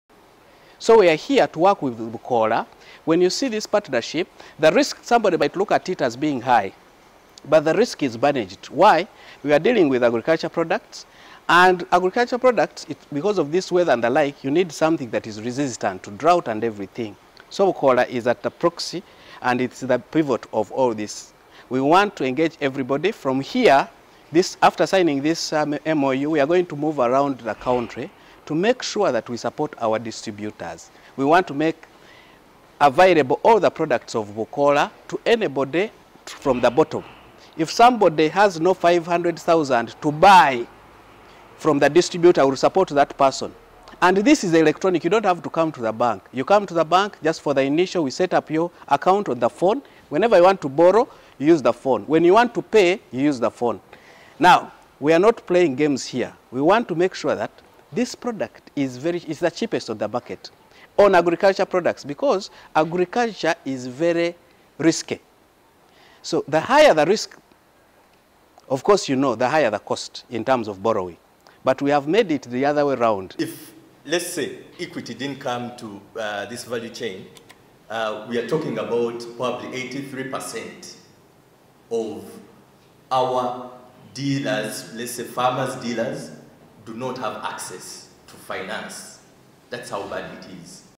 FUNDING AGRO-INPUTS: Equity bank partners with Bukoola chemicals
One of the sticking issues in the agro-input area of agriculture is counterfeit or substandard supplies. On Tuesday, Equity Bank partnered with a local agro-input maker and distributor, Bukoola, to find quick solutions to cancerous fake inputs for animal and crop husbandry on the market today.